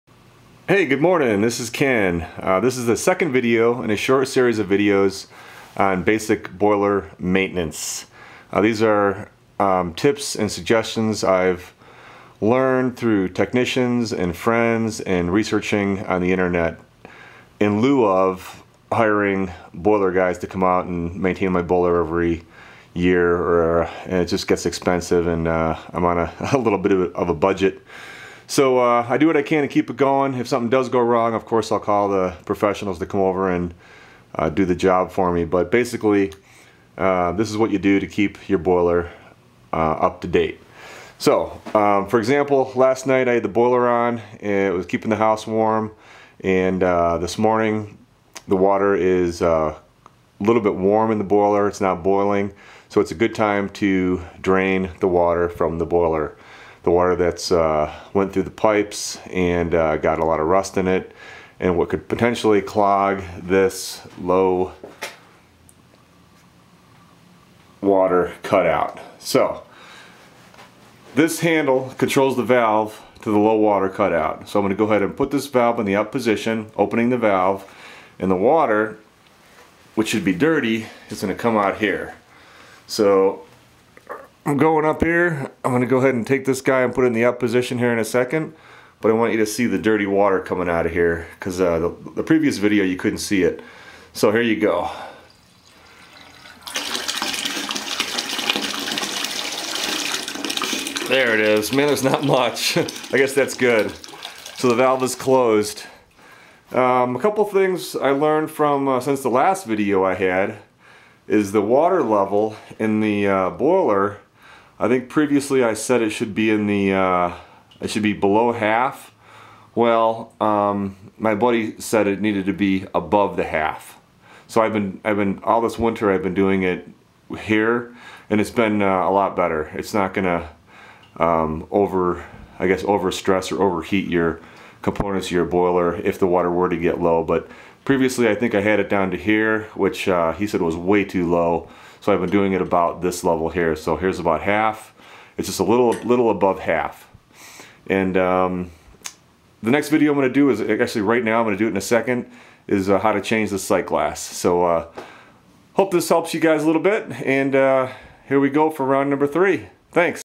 Everyday Boiler Maintenance
How to drain your low water cut out from pipe residue. How to adjust your water level in the boiler using the site-glass. Clean the water out every couple days with heavy use! Just let it run until its clean, then replenish it to the desired level.